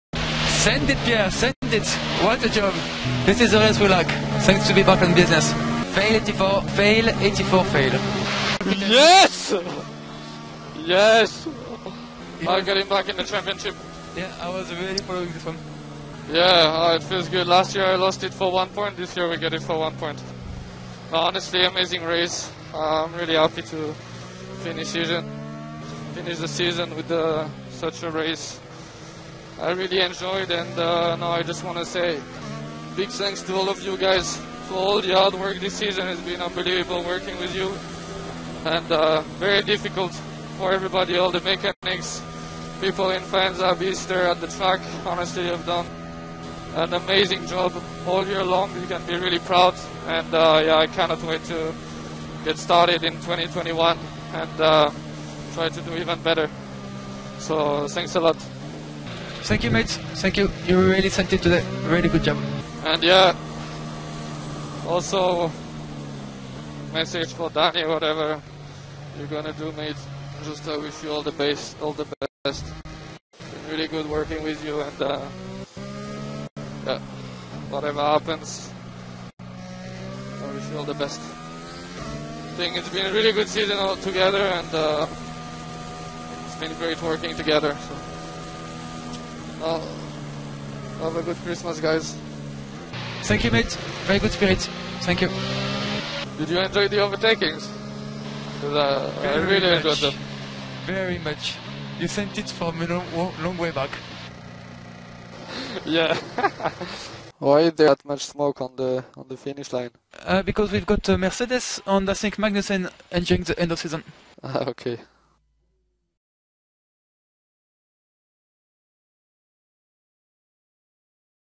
【F1無線】口を滑らせた？ガスリーがグビアトに意味深メッセージ「I wish you all the best」｜2020 アブダビGP｜F1で学ぶ英会話
角田のF1デビューが発表されましたね！実はアブダビでのレース後、ガスリーはクビアトへ意味深なメッセージを送ります。どうやら知っていたようですね(笑)

●【無線で学ぶF1週末英会話】アブダビGP「さようなら、そして”All the best”」↓

※This video is for entertainment and educational purpose only. All rights for radio footages are property on FOM. The purpose of this video is for Japanese fans to enjoy F1 more.